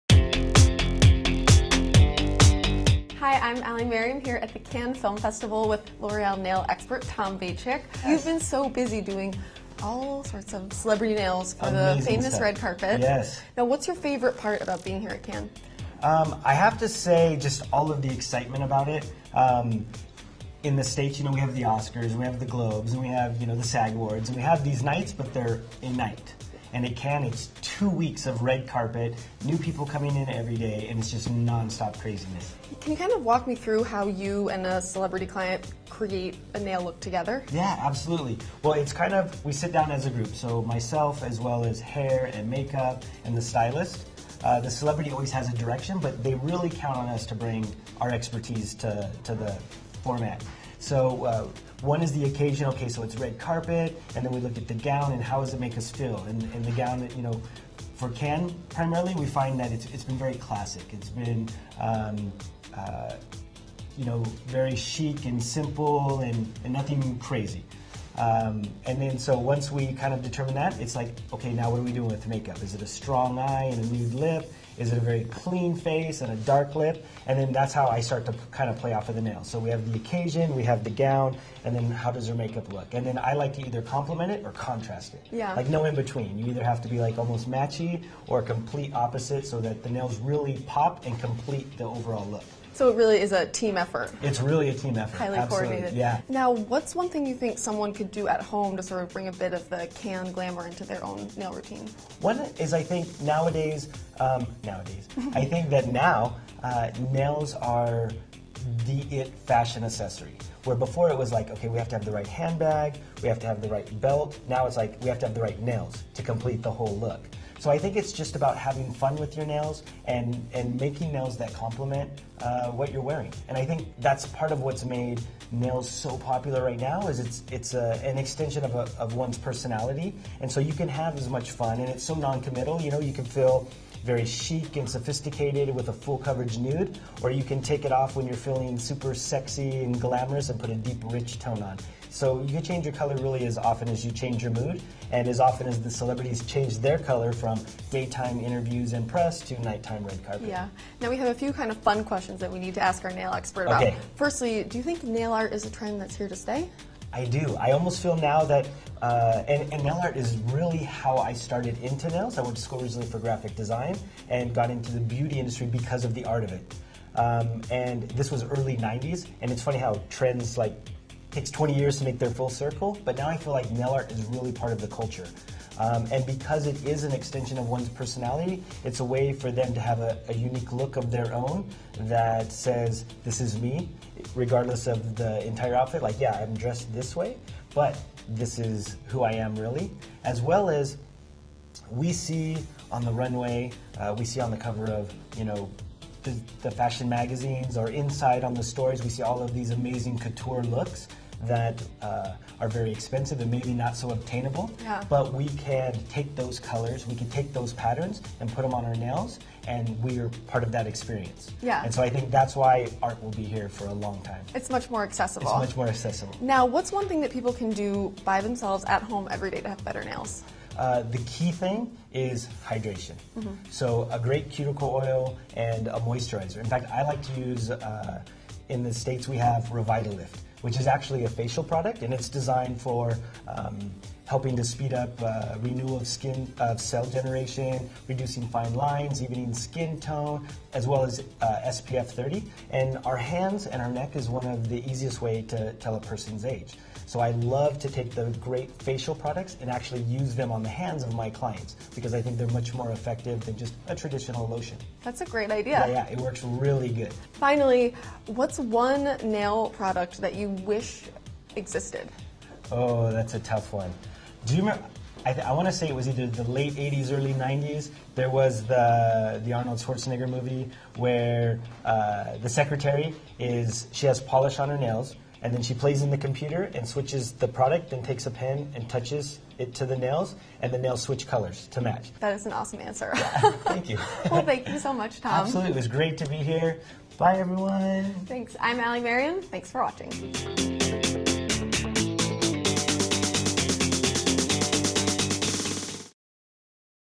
Manicure Tips From Zooey Deschanel's Nail Artist Tom Bachik | Beauty Beat
L'Oréal Paris nail expert Tom Bachik gave us the inside scoop on getting glamorous nails. We caught up with Tom earlier at the Cannes Film Festival, where he was busy working with the beauty company's ambassadors Doutzen Kroes, Eva Longoria, and Jane Fonda! Tom filled us in on how to get Cannes-level glamour at home and also shared how it's possible — and easy — to add pampering to your at-home manicure routine. On Allie: H&M jacket and Zara top.